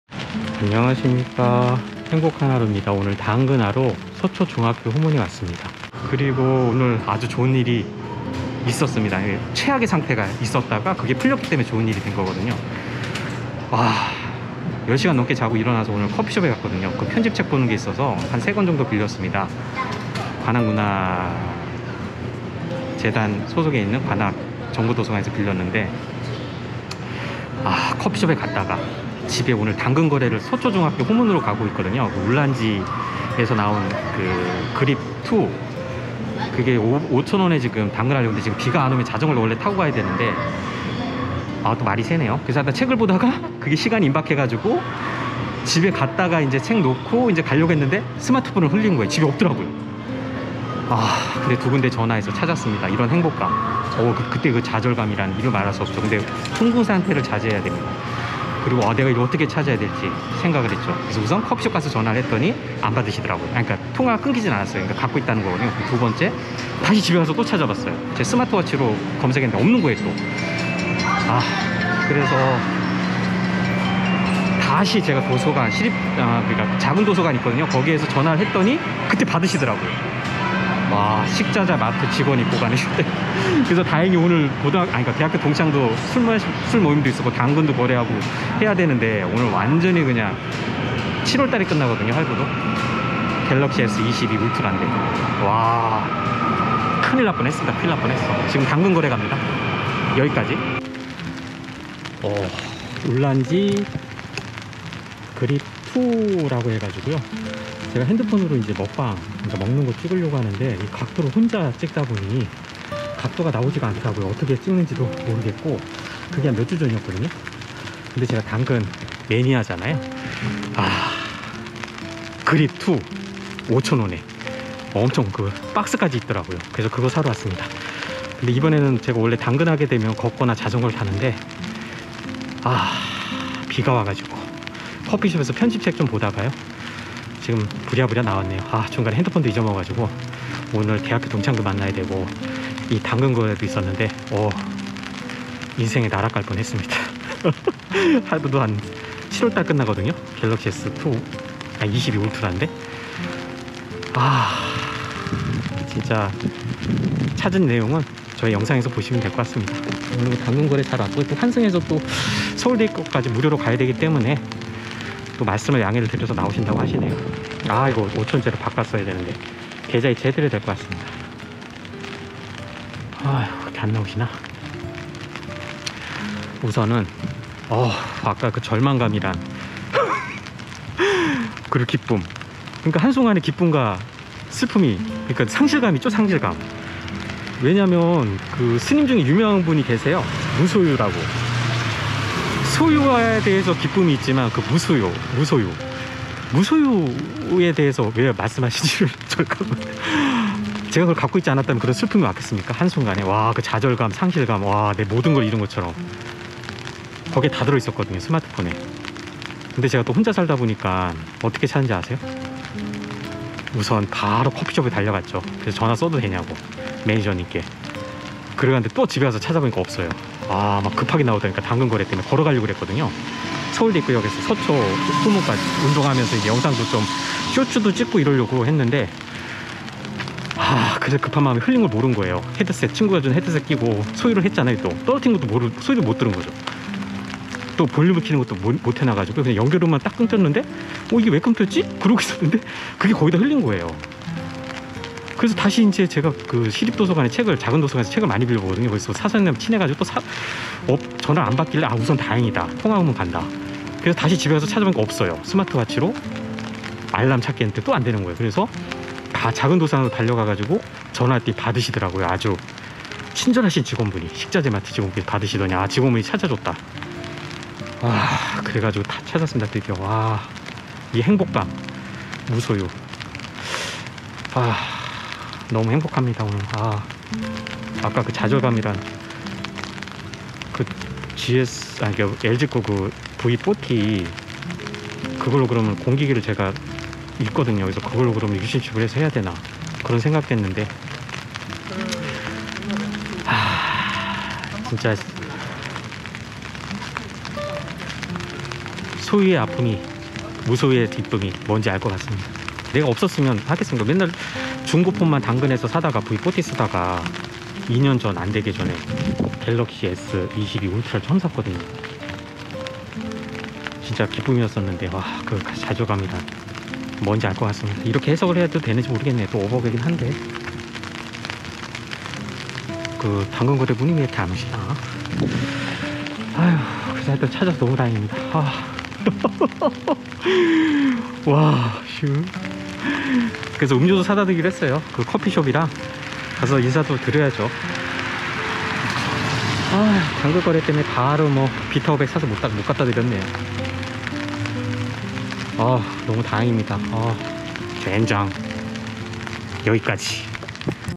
20240221당근거래 출발전 1/2 무소유란! 누구든지 소유가 필요하지만 삶에 있어서 크나큰 좌절감을 줄수도 있다는 것을 ~~~너무 없다고 슬프지 않는다
오늘 성철 스님의 무소유에 대해서 알게 해준 일이 있었다 그것은 할부가 남아있는 스마트폰을 분실하였다는 것이다.

1. 울란지 그립 2  당근거래  당근거래가역 5,000원

2. 네이버 이동 경로

걷기로 하려다고 휴대폰 분실 상황이라는 긴급한 상황이 발생하여  환승통한 대중교통을 통해서 이동한 궤적으로 뵈주시면 될 것 같습니다.

환승 경로 : 봉천역에서 지하철타고 서초역에서 내려 서초역에서 마을버스 02룰 타고 서초유스센터 하차함 그리고 서초중학교 후문으로 이동하여 당근거래후  거래자님이 늦게 나오심 거래끝나자 10분 남음 열라게 서초유스센터로 뛰어가서 5413버스를 다시 타고 서울대입구역에서 내림 그래서 환승으로 당근거래 가성비 있게 종료

3.Ulanzi MA35 휴대폰 카메라 그립 핸들, 맥세이프 블루투스 셔터, 스마트폰 셀카 안정기, 수직 수평 촬영  이 기능이 더 좋은데요. 12392원이네요. ㅋㅋㅋㅋㅋ